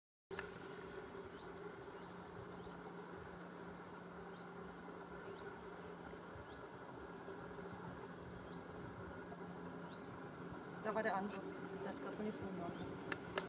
Wuhletal 23.07.22 01 Eisvogel
Ein wundervoller Tag in Wuhletal, wo wir eine Biberfamilie begegnet sind